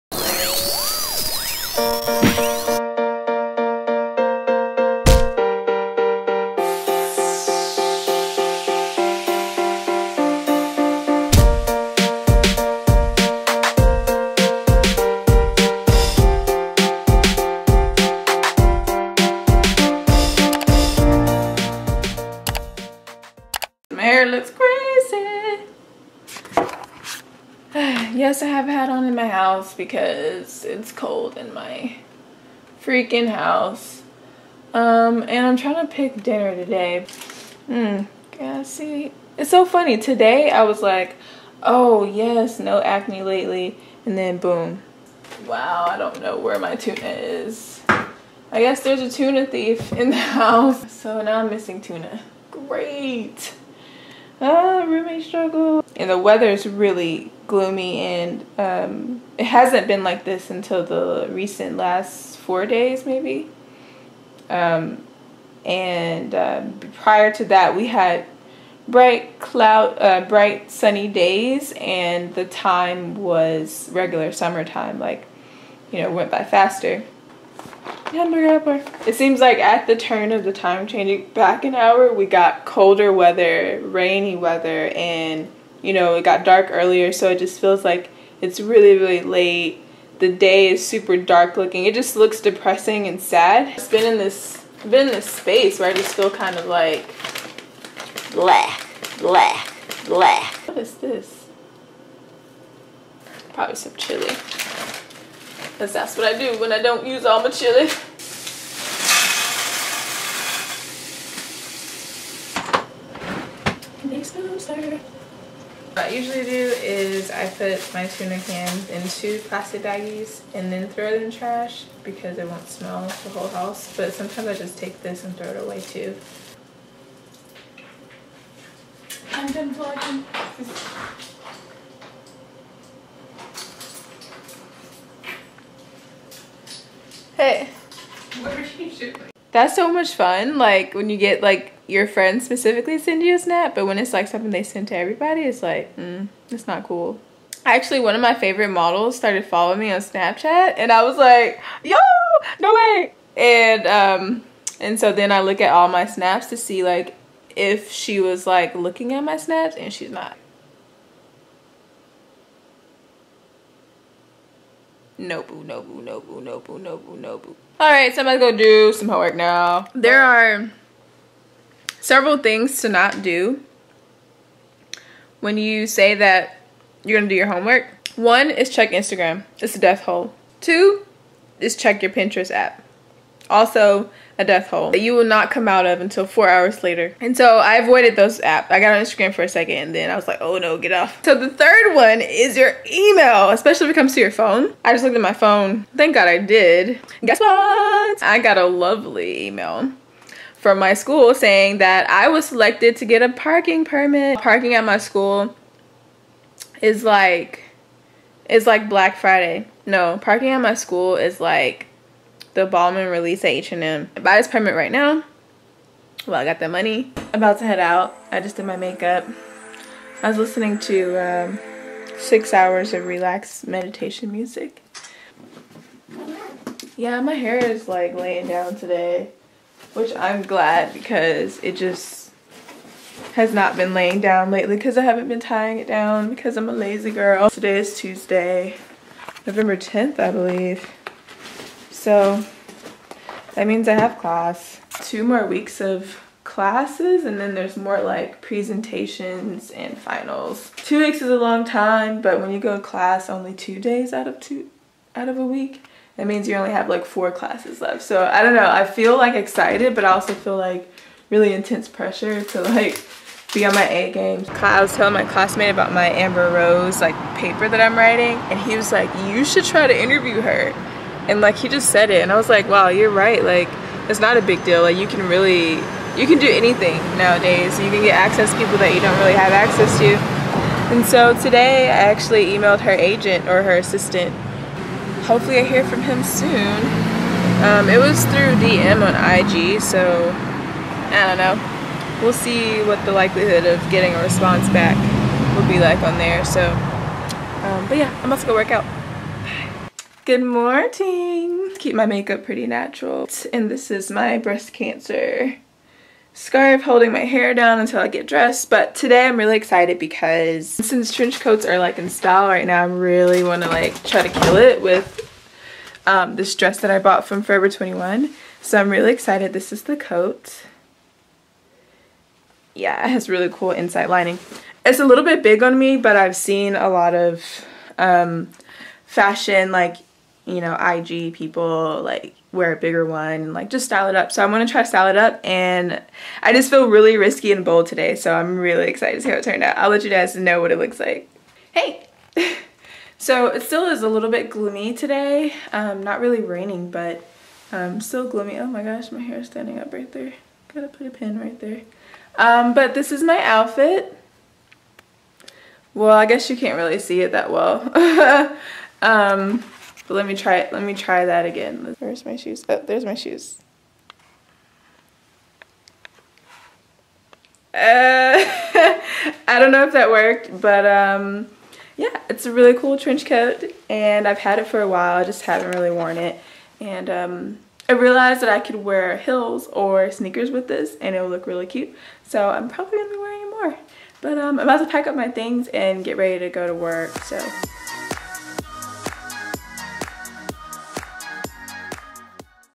Vlog | 11.13 - Trench Coats, GSU Parking, & Food
This week I styled up my wardrobe a bit, beat my face a few times and finally had time to cook an actual meal! Also, I started listening to smooth meditation music in the mornings to start off my day. Give it a try! You may notice less stress in your week.